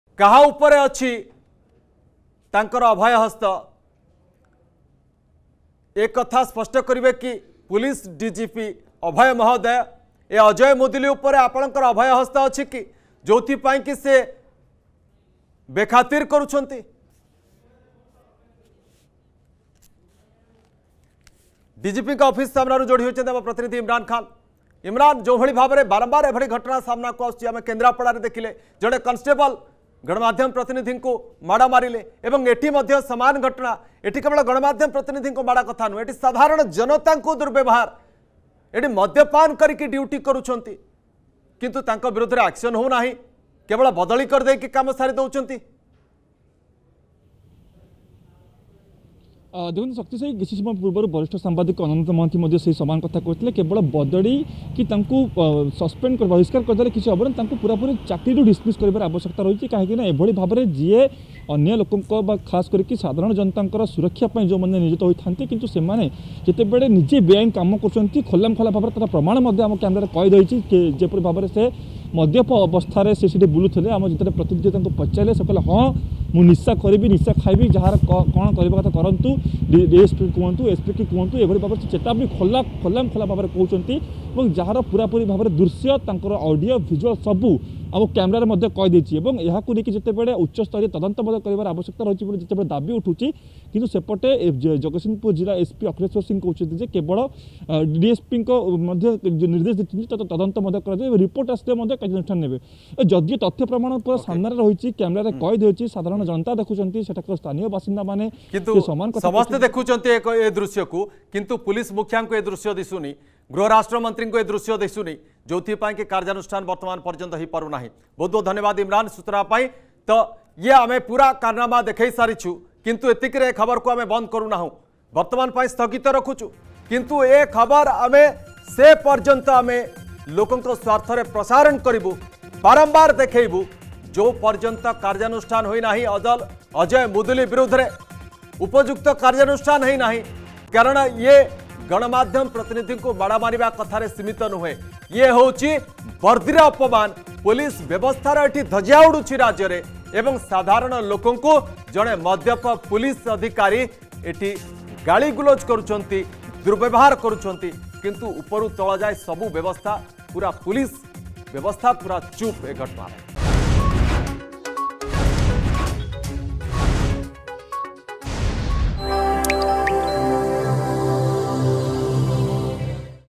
ପୋଲିସ ବଦ୍ଦିରେ ମଦ
ମଦ ପିଇ ଲୋକଙ୍କ ଉପରେ ଦାଦାଗିରି ଦେଖାଇବା ସହ ଅର୍ଗସ ପ୍ରତିନିଧି ଙ୍କୁ  ଠେଲାପେଲା କରି ମାଡ ମାରିଛନ୍ତି ବନ୍ଦର ମେରାଇନ ଥାନା ଆଇଆଇସି ଅଜୟ ମୁଦୁଲି